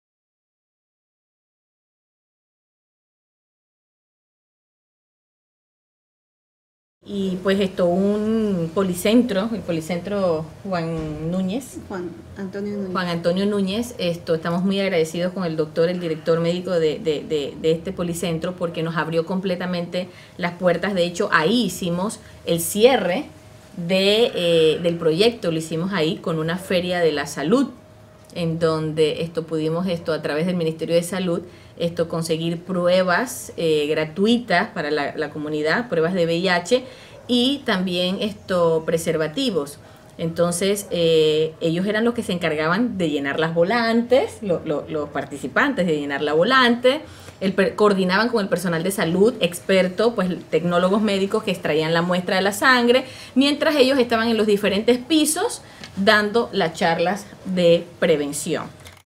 Proyecto Afrolatin@ presenta a Conversemos VIH/SIDA (Feria de Salud/Conclusion de la Investigación)
Equipo de investigación sobre la conclusión del proyecto.

Investigative team on the conclusion of the project.